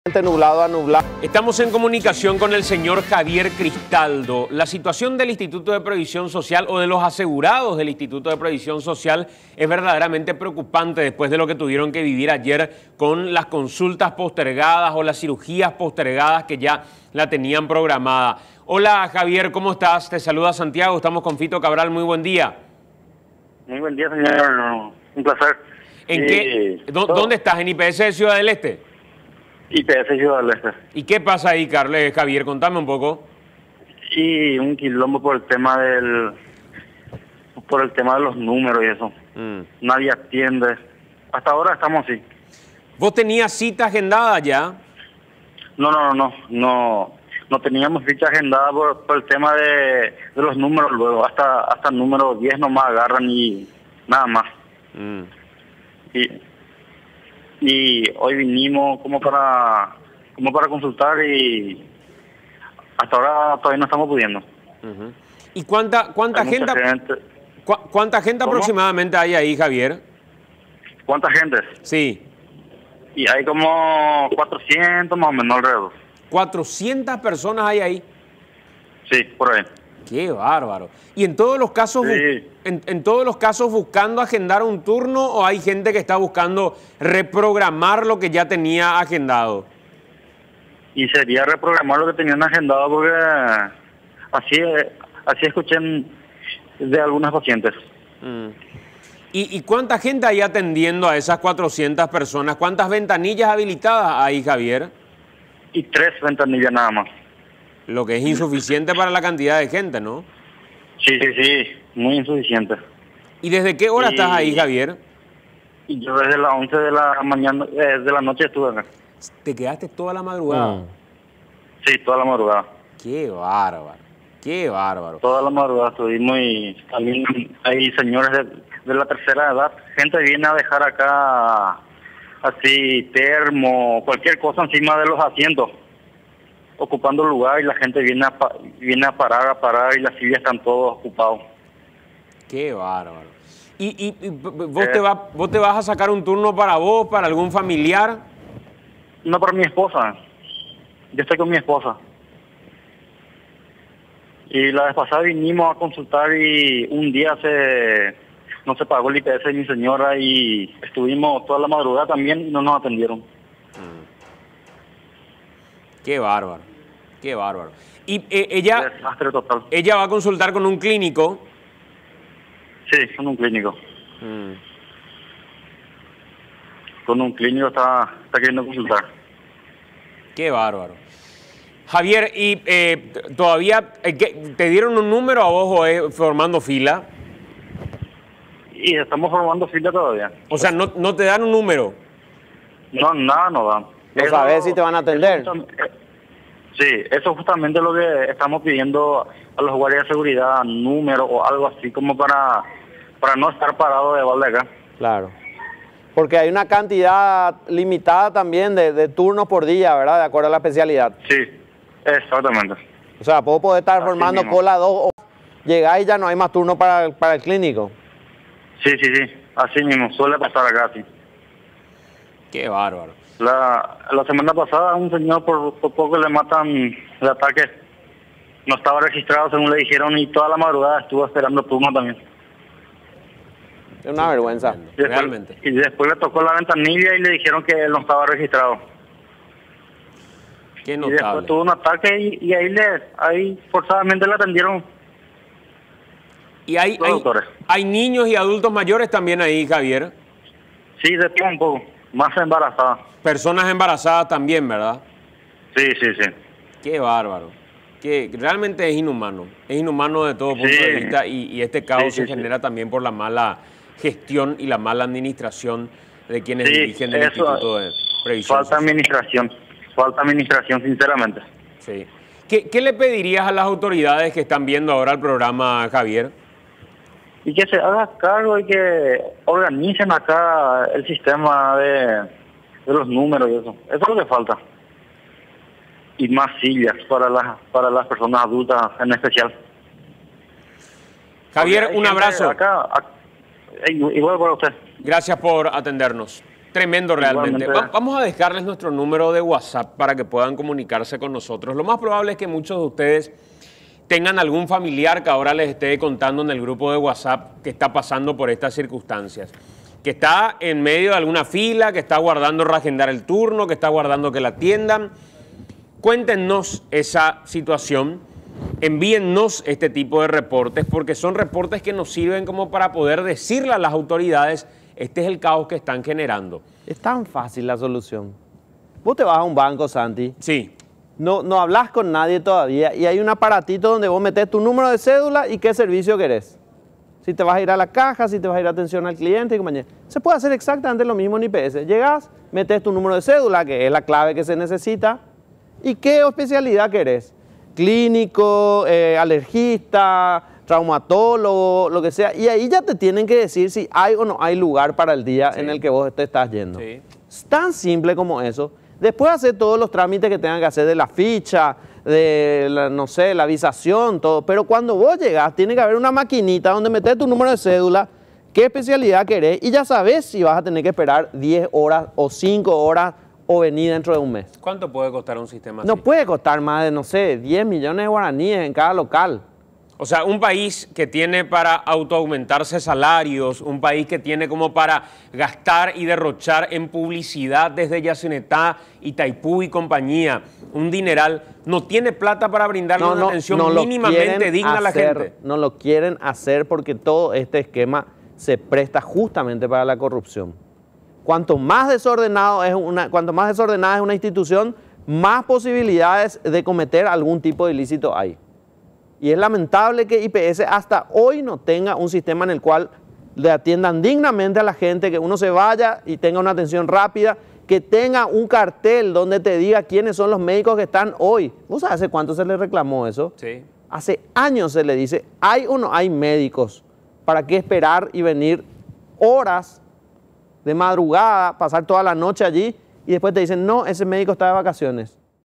Pacientes aguardan turno desde la medianoche en IPS de Ciudad del Este
Asegurados del IPS de Ciudad del Este forman filas interminables desde la medianoche para poder reagendar su cita del día anterior. Denuncian desorden de funcionarios a la hora de entregar un número para consultas.